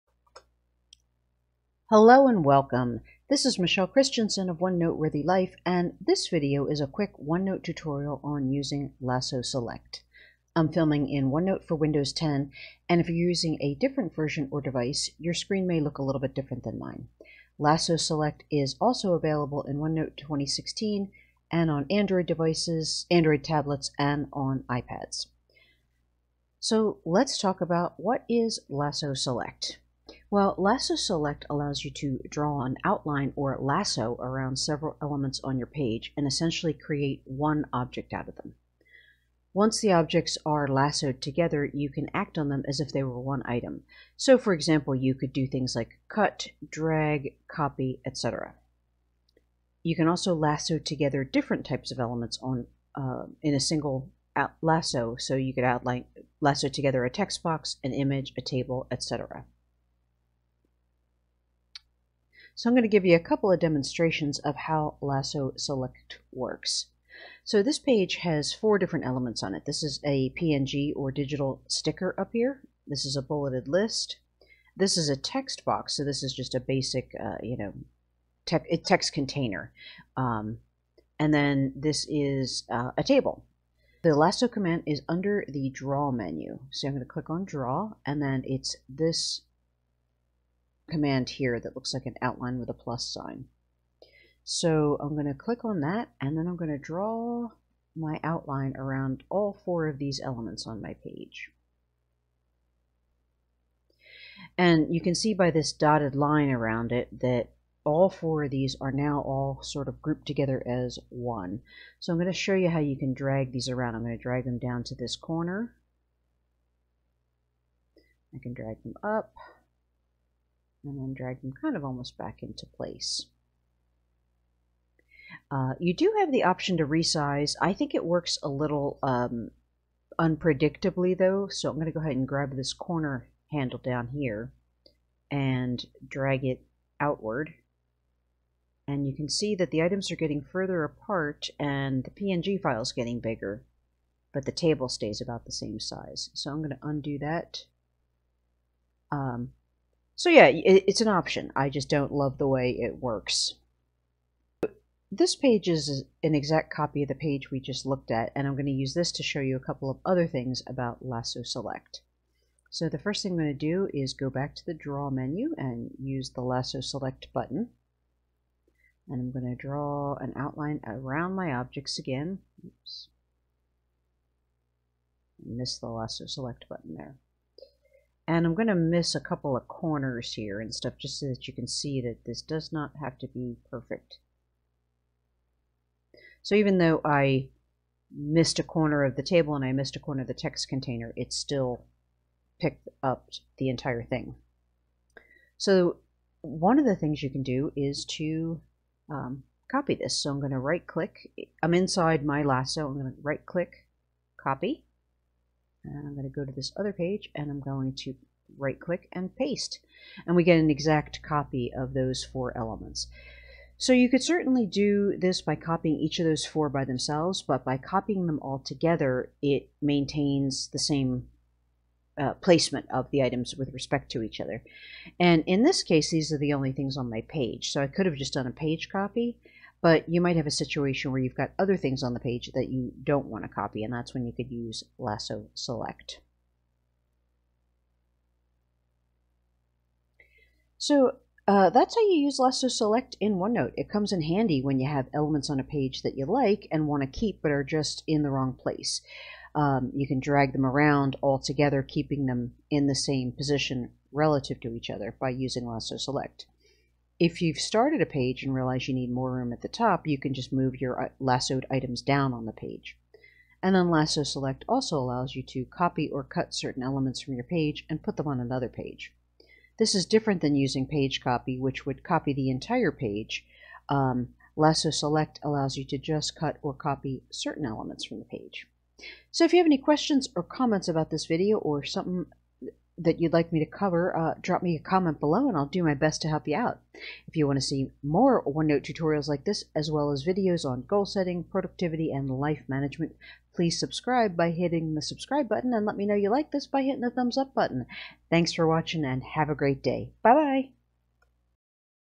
Using Lasso Select in OneNote
This is a quick tutorial on a handy OneNote feature called Lasso Select. This feature allows you to "lasso" together separate elements on your page and treat them like they are one object. Once lassoed together, you can move, copy, or cut and past the entire group of objects as one unit.